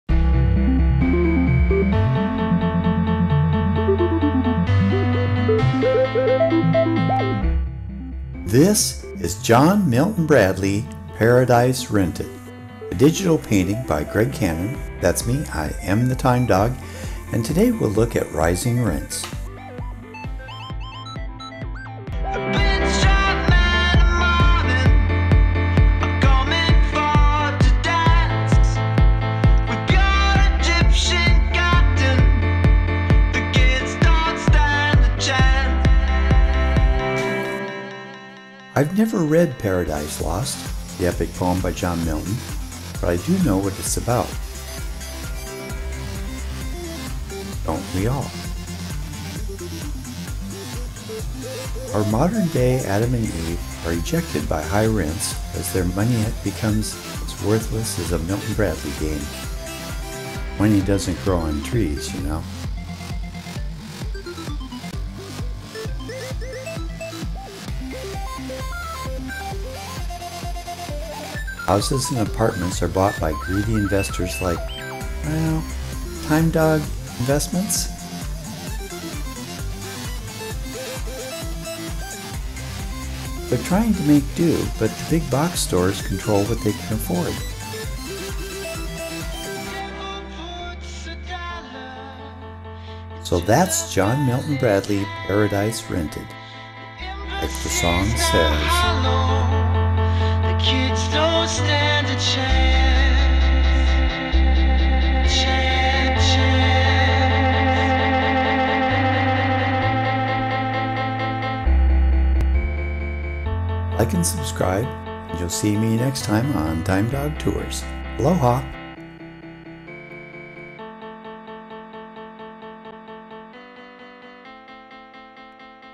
John Milton-Bradley: Paradise Rented
Video walkthrough of my latest painting, "John Milton-Bradley: Paradise Rented", a word play on John Milton and the makers of Monopoly. "Paradise Lost" updated for 21st century.
Visit timedogtours.com for my portfolio site.